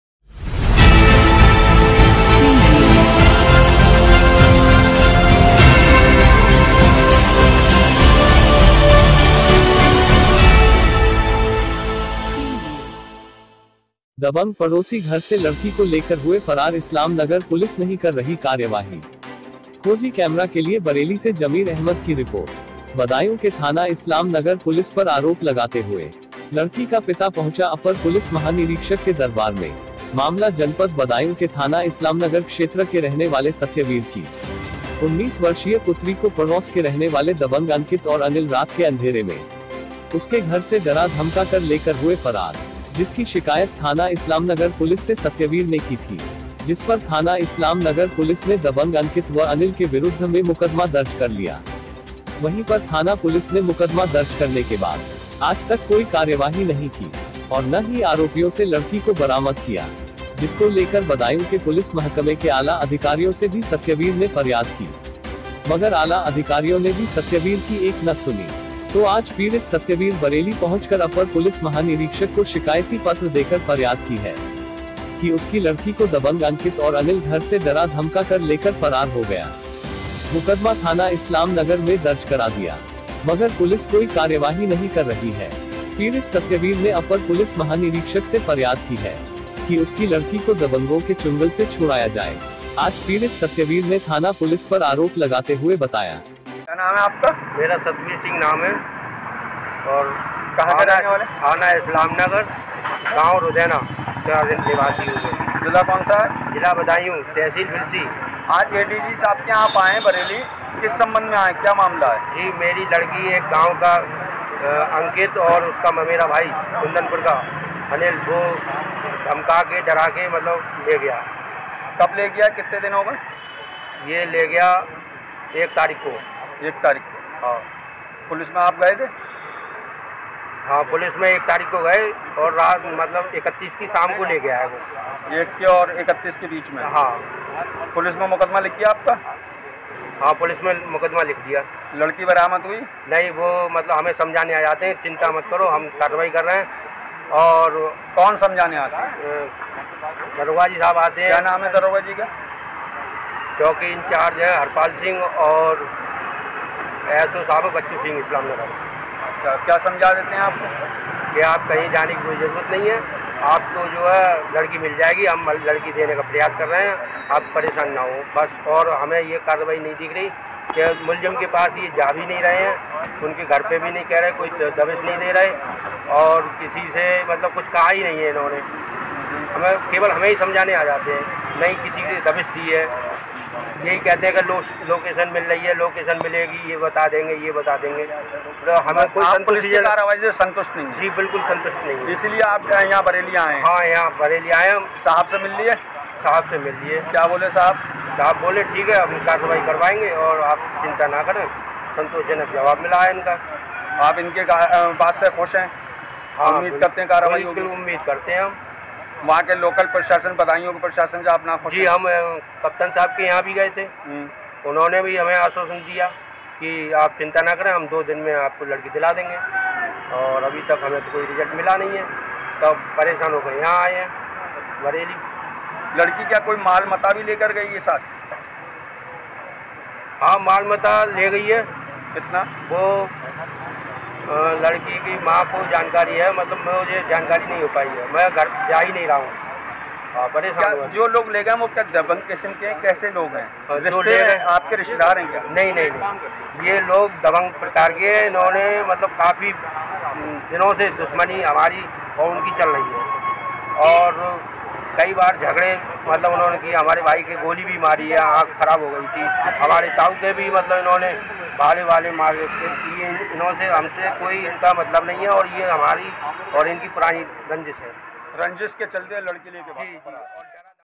दबंग पड़ोसी घर से लड़की को लेकर हुए फरार इस्लाम नगर पुलिस नहीं कर रही कार्यवाही।KHOJI CAMERA
KHOJI CAMERA दबंग पड़ोसी घर से लड़की को लेकर हुए फरार इस्लाम नगर पुलिस नहीं कर रही कार्यवाही।